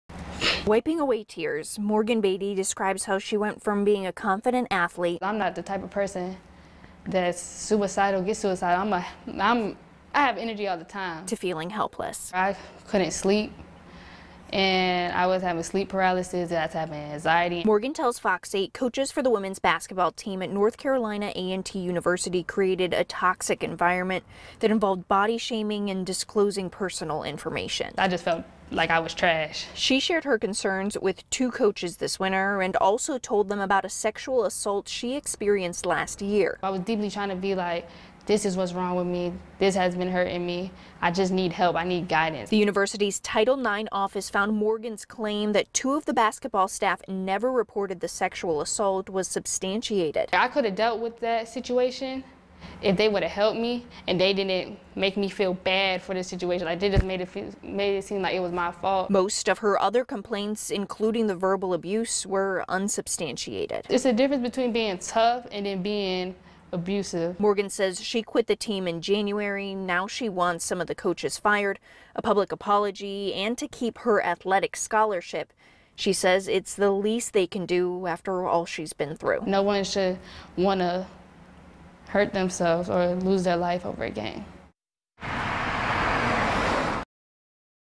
Former A&T women`s basketball player says coaches failed to report a sexual assault, created a toxic
A former A&T women's basketball player says coaches at the university failed to report a sexual assault and created a toxic environment.